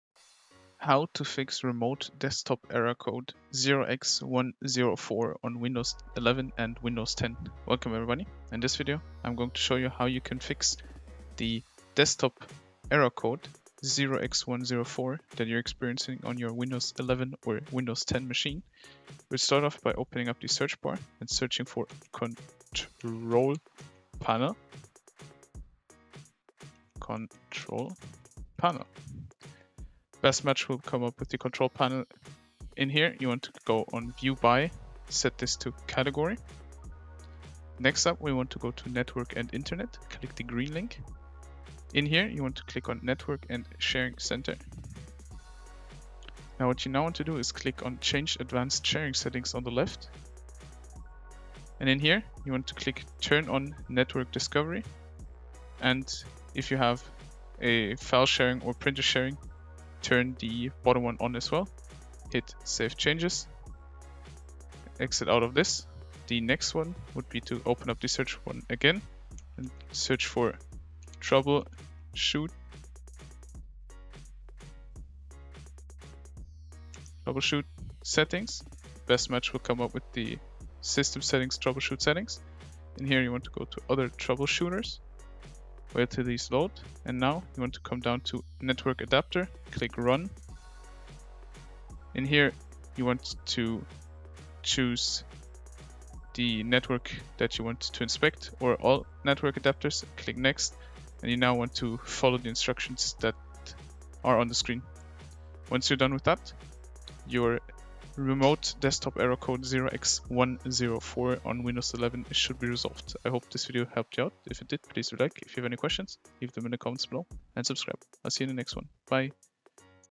Fix Remote Desktop Error Code 0x104 on Windows 11/10
Remote Desktop Protocol is a built-in Windows utility that you can use to access computers over the same or different networks without having physical access. This is useful as you are allowed to control the target system using a few clicks from your office or home. There are tons of third-party RDP utilities out there. The built-in RDP might not “really” be the best off of them; most users still prefer to use it. Of course, there are some issues with it as well, like any other software. The one we are going to discuss in this tutorial is the error code 0x104.

This error message pops up when you are trying to connect to the target system on the same or a different network. The error message states “We couldn’t connect to the remote PC because the PC can’t be found. Please provide the fully-qualified name or the IP address of the remote PC, and then try again”. This can be due to your DNS settings, network discovery options or occasionally, the Windows defender firewall privacy settings.

A number of users have reported seeing the Remote Desktop error 0x104 on Windows 10. In some cases, the connection works only when both of the computers are very close to each other.

This can be an extremely frustrating problem, as many people around the world need to access their work servers remotely and they use this feature to collect or access data from the company they work at.

In some other cases, users that have tried connecting to their remote servers have also observed their Internet connection fails.

Issues addressed in this tutorial:
we couldn't connect to the remote pc error code 0x104
we couldn't connect to the remote pc error code 0x204